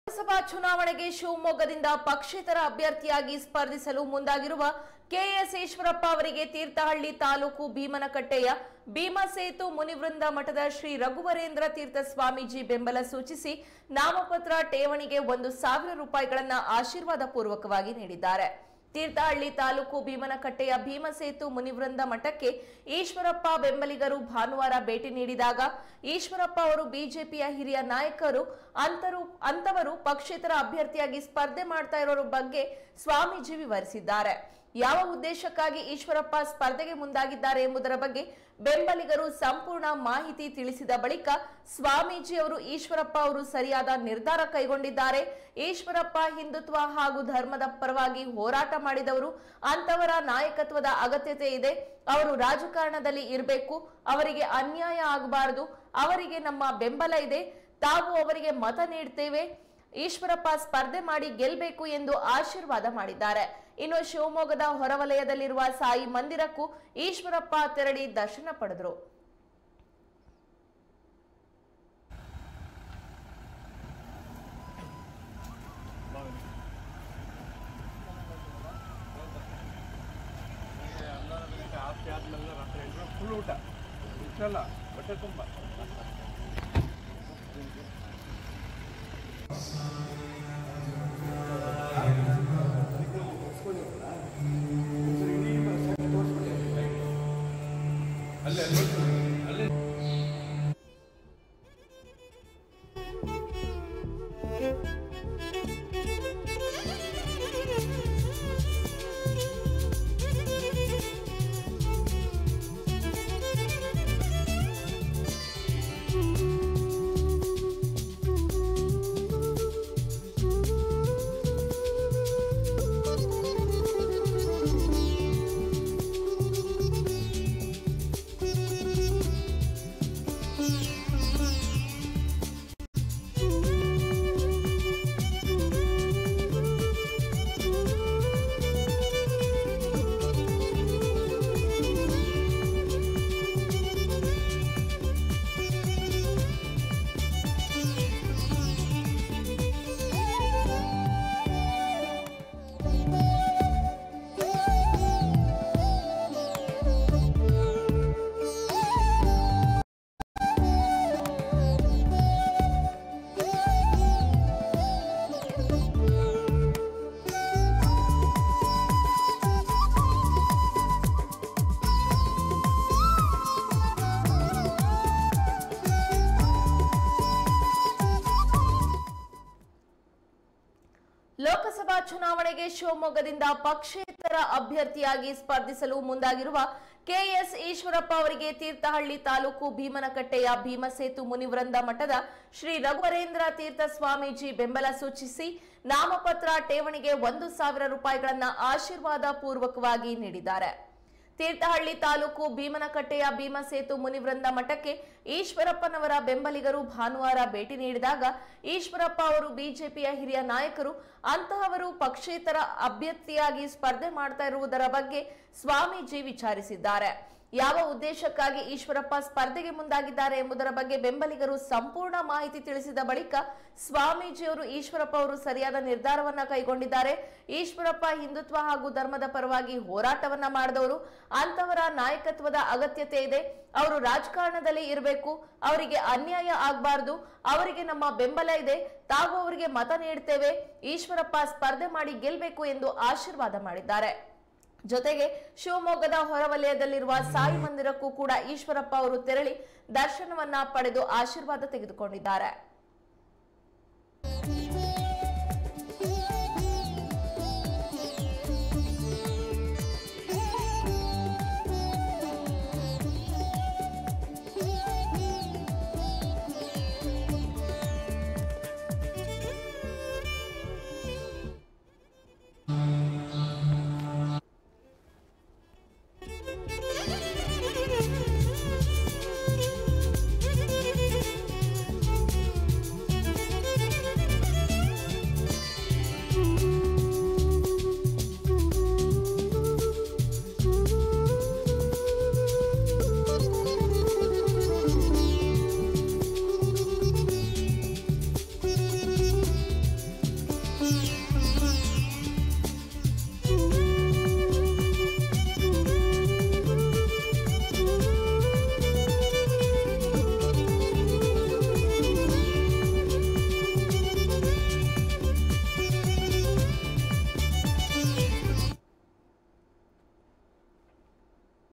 BJP ಬಂಡಾಯ ಅಭ್ಯರ್ಥಿಯಾಗಿ KS Eshwarappa ಸ್ಪರ್ಧೆ | ಭೀಮನಕಟ್ಟೆ ಶ್ರೀಗಳಿಂದ ಆಶೀರ್ವಾದ
ಲೋಕಸಭಾ ಚುನಾವಣೆಗೆ ಶಿವಮೊಗ್ಗದಿಂದ ಪಕ್ಷೇತರ ಅಭ್ಯರ್ಥಿಯಾಗಿ ಸ್ಪರ್ಧಿಸಲು ಮುಂದಾಗಿರುವ ಕೆ.ಎಸ್.ಈಶ್ವರಪ್ಪ ಅವರಿಗೆ ತೀರ್ಥಹಳ್ಳಿ ತಾಲೂಕು ಭೀಮನಕಟ್ಟೆಯ ಭೀಮಸೇತು ಮುನಿವೃಂದ ಮಠಜ ಶ್ರೀ ರಘುವರೇಂದ್ರ ತೀರ್ಥ ಸ್ವಾಮೀಜಿ ಬೆಂಬಲ ಸೂಚಿಸಿ, ನಾಮಪತ್ರ ಠೇವಣಿಗೆ ಒಂದು ಸಾವಿರ ರು.ಗಳನ್ನು ಆಶೀರ್ವಾದ ಪೂರ್ವಕವಾಗಿ ನೀಡಿದ್ದಾರೆ. ತೀರ್ಥಹಳ್ಳಿ ತಾಲೂಕು ಭೀಮನಕಟ್ಟೆಯ ಭೀಮಸೇತು ಮುನಿವೃಂದ ಮಠಕ್ಕೆ ಈಶ್ವರಪ್ಪ ಬೆಂಬಲಿಗರು ಭಾನುವಾರ ಭೇಟಿ ನೀಡಿದಾಗ, ಈಶ್ವರಪ್ಪ ಅವರು ಬಿಜೆಪಿಯ ಹಿರಿಯ ನಾಯಕರು ಅಂತವರು ಪಕ್ಷೇತರ ಅಭ್ಯರ್ಥಿಯಾಗಿ ಸ್ಪರ್ಧೆ ಮಾಡುತ್ತಿರುವುದೇ ಎಂದು ಬಗ್ಗೆ ಸ್ವಾಮೀಜಿ ವಿಚಾರಿಸಿದ್ದಾರೆ.  ಯಾವ ಉದ್ದೇಶಕ್ಕಾಗಿ ಈಶ್ವರಪ್ಪ ಸ್ಪರ್ಧೆಗೆ ಮುಂದಾಗಿದ್ದಾರೆ ಎಂಬುದರ ಬಗ್ಗೆ ಬೆಂಬಲಿಗರು ಸಂಪೂರ್ಣ ಮಾಹಿತಿ ತಿಳಿಸಿದ ಬಳಿಕ ಸ್ವಾಮೀಜಿಯವರು ಈಶ್ವರಪ್ಪ ಅವರು ಸರಿಯಾದ ನಿರ್ಧಾರ ಕೈಗೊಂಡಿದ್ದಾರೆ. ಈಶ್ವರಪ್ಪ ಹಿಂದುತ್ವ ಹಾಗೂ ಧರ್ಮದ ಪರವಾಗಿ ಹೋರಾಟ ಮಾಡಿದವರು. ಅಂತವರ ನಾಯಕತ್ವದ ಅಗತ್ಯತೆ ಇದೆ. ಅವರು ರಾಜಕಾರಣದಲ್ಲಿ ಇರಬೇಕು. ಅವರಿಗೆ ಅನ್ಯಾಯ ಆಗಬಾರದು. ಅವರಿಗೆ ನಮ್ಮ ಬೆಂಬಲ ಇದೆ. ತಾವು ಅವರಿಗೆ ಮತ ನೀಡುತ್ತೇವೆ. ಈಶ್ವರಪ್ಪ ಸ್ಪರ್ಧೆ ಮಾಡಿ ಗೆಲ್ಲಬೇಕು ಎಂದು ಆಶೀರ್ವಾದ ಮಾಡಿದ್ದಾರೆ. ಇನ್ನು ಶಿವಮೊಗ್ಗದ ಹೊರ ವಲಯದಲ್ಲಿರುವ ಸಾಯಿ ಮಂದಿರಕ್ಕೂ ಈಶ್ವರಪ್ಪ ತೆರಳಿ ದರ್ಶನ ಪಡೆದರು.